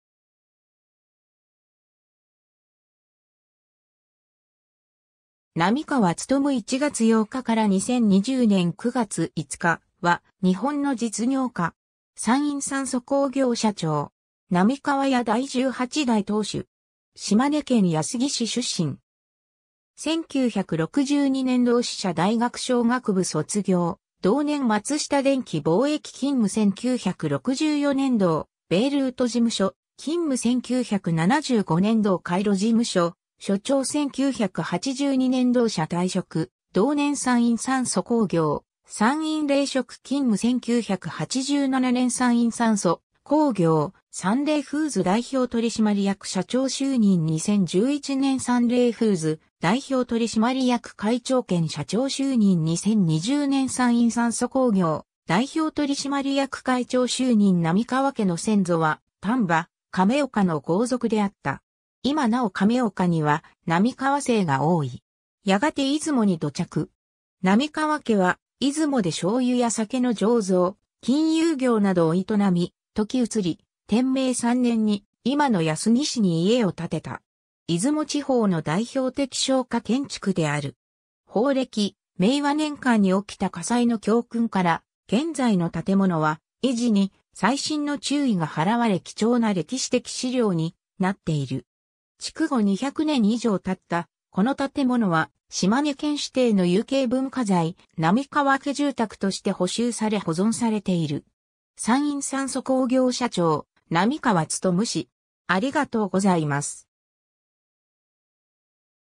並河勉
並河 勉（なびか つとむ、1939年（昭和14年）1月8日 -2020年（令和2年）9月5日 ）は日本の実業家。
山陰酸素工業社長。
並河家第18代当主。
 島根県安来市出身。
 1962年（昭和37年）同志社大学商学部卒業、同年松下電器貿易勤務 1964年（昭和39年）同ベイルート事務所勤務 1975年（昭和50年）同カイロ事務所所長 1982年（昭和57年）同社退職、同年山陰酸素工業、山陰冷食（現さんれいフーズ）勤務 1987年（昭和62年）山陰酸素工業・さんれいフーズ代表取締役社長就任 2011年（平成23年）さんれいフーズ 代表取締役会長兼社長就任 2020年（令和 2年）山陰酸素工業代表取締役会長就任 並河家の先祖は丹波・亀岡の豪族であった。
今なお亀岡には並河姓が多い。
やがて出雲に土着。
並河家は出雲で醤油や酒の醸造、金融業などを営み、時移り天明3年（1783年）に今の安来市に家を建てた。
出雲地方の代表的商家建築である。
宝暦・明和年間（1700年代中ごろ）におきた火災の教訓から現在の建物は維持に細心の注意が払われ貴重な歴史的資料になっている。
築後二百年以上経ったこの建物は、島根県指定の有形文化財「並河家住宅」として補修され保存されている。
 ^ 山陰酸素工業社長 並河勉氏